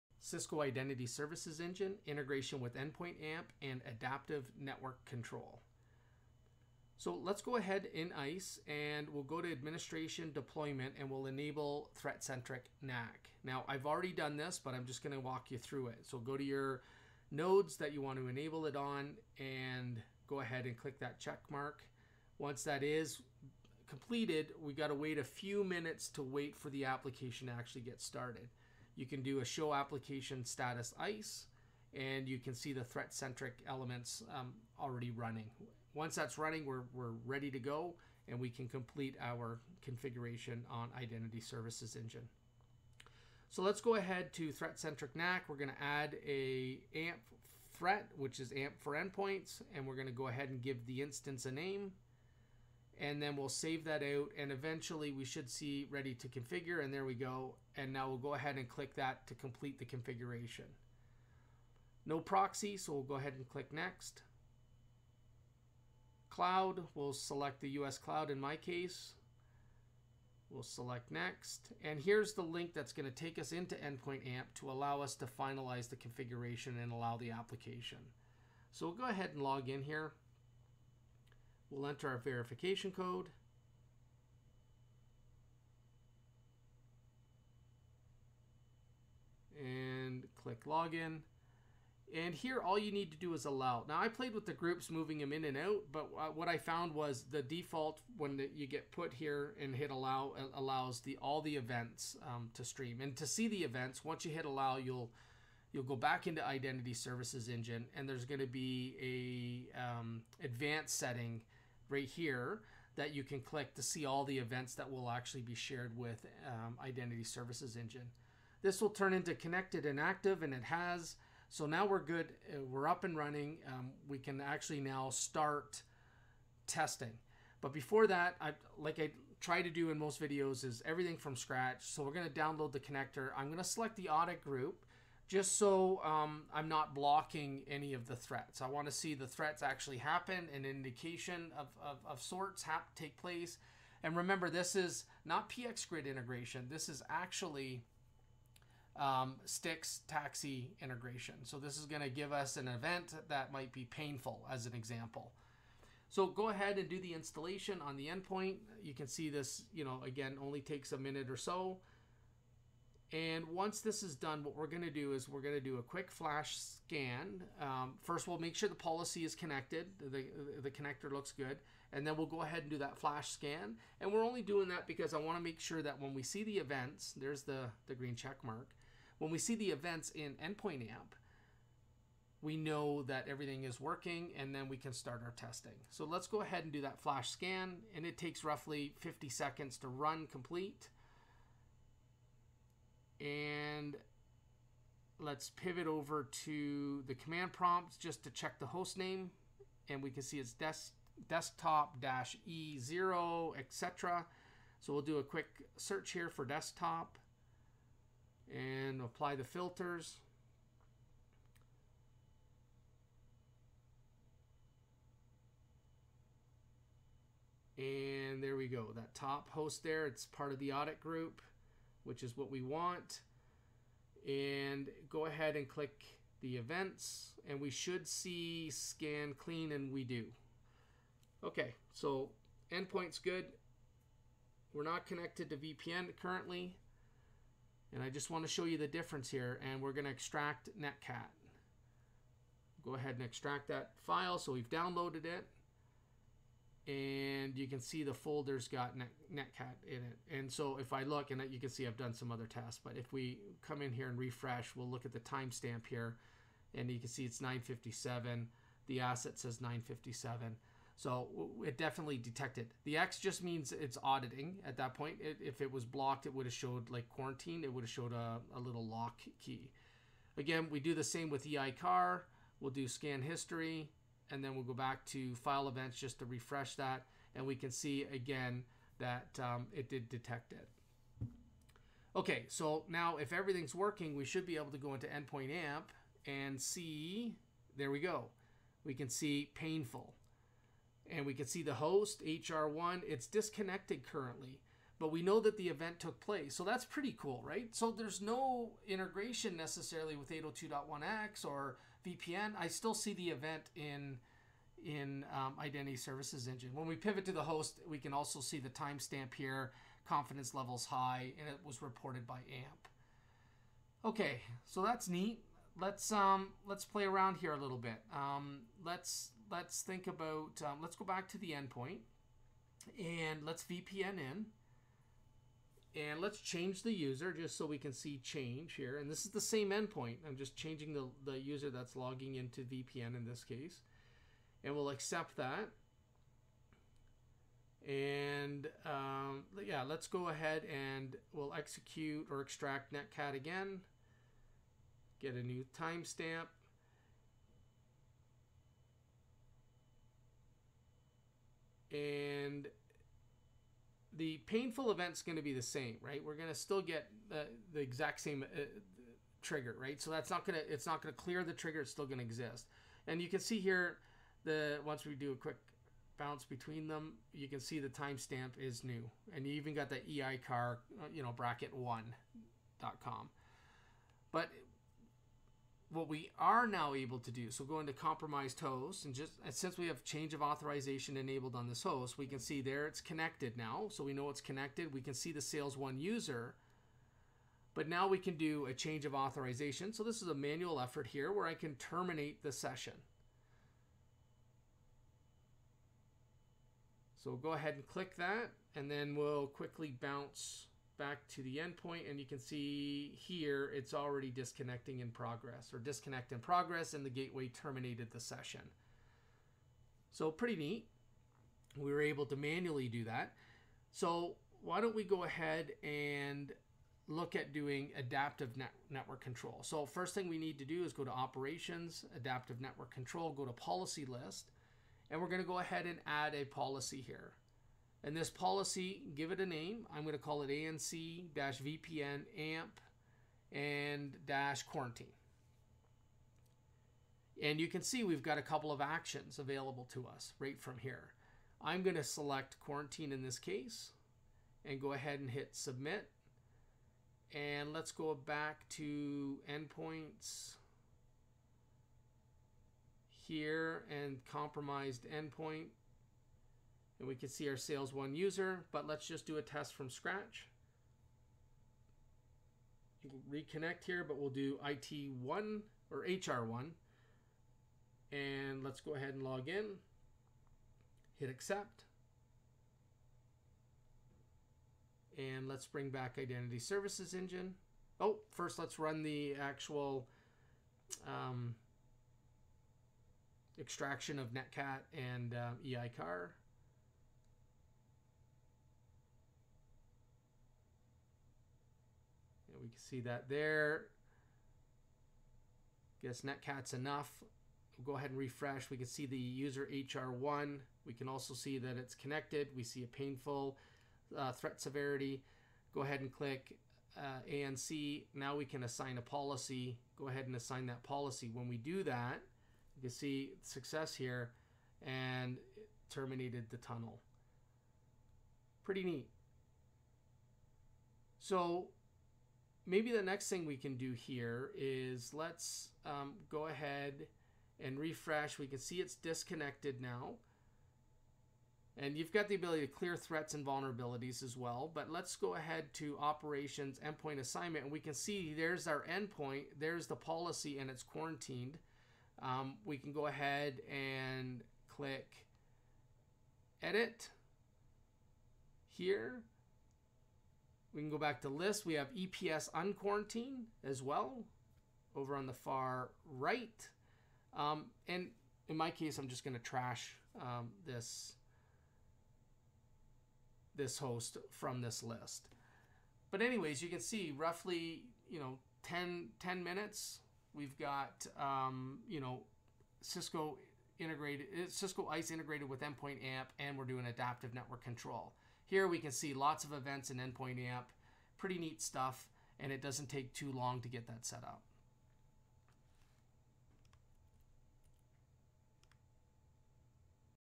Cisco ISE: Integration with Endpoint AMP Leveraging ANC
In this video we will leverage Threat Centric NAC and endpoint AMP using Adaptive Network Control (ANC). The integration uses STIX and TAXII and provides the ability to further action against threats. This will include a couple of test use cases as well.